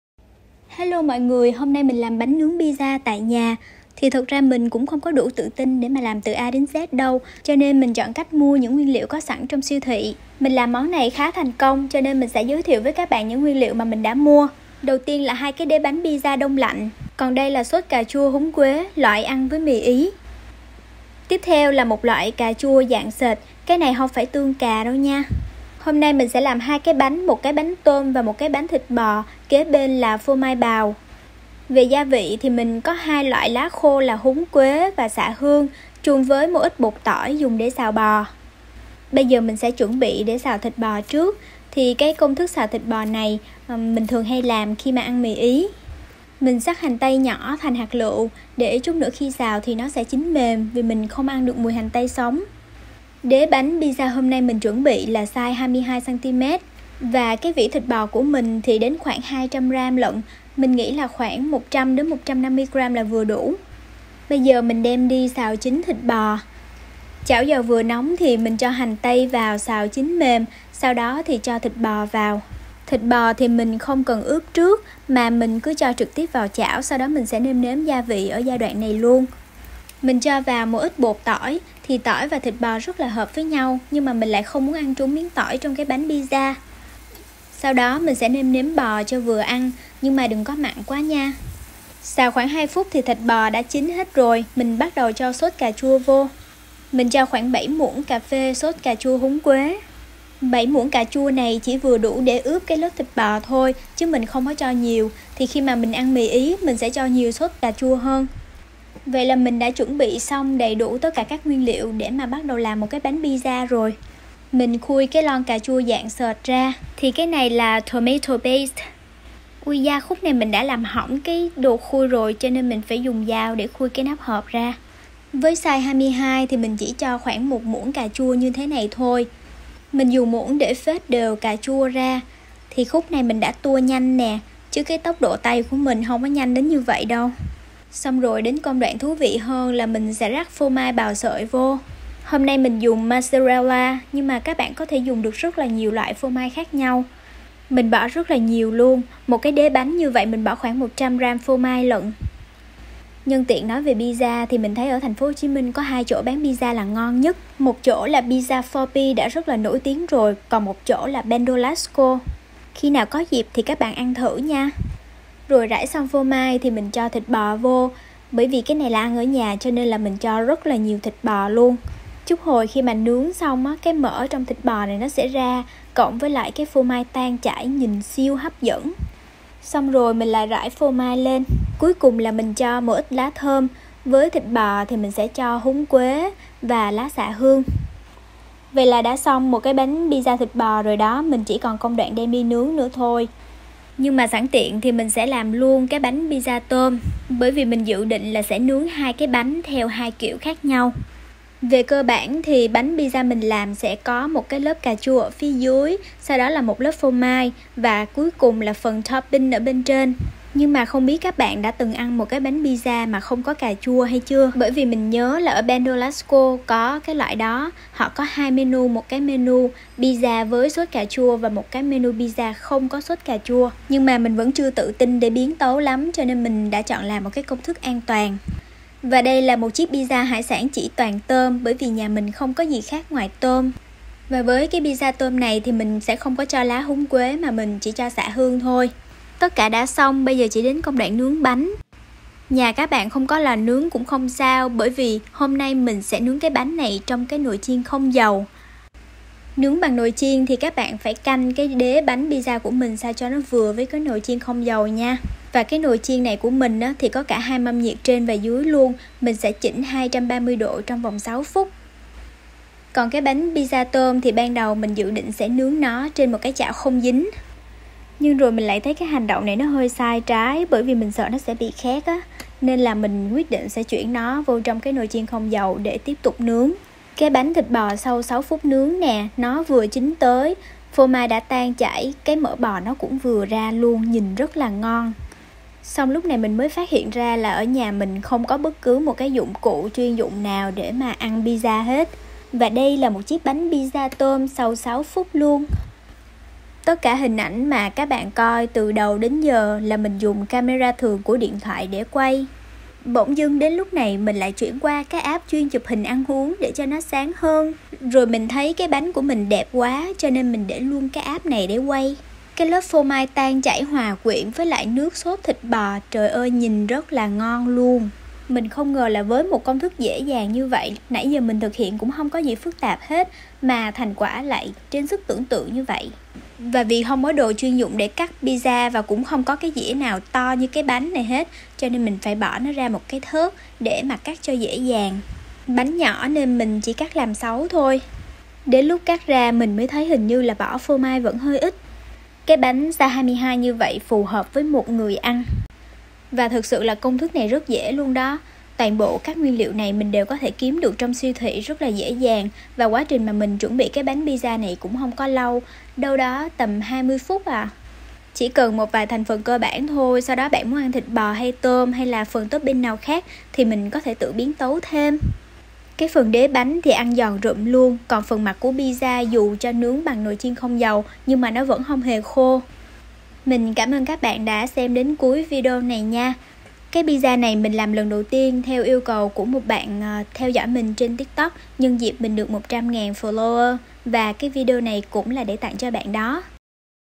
Làm pizza đơn giản tại nhà
Tự làm bánh pizza tại nhà để ăn cho đỡ thèm .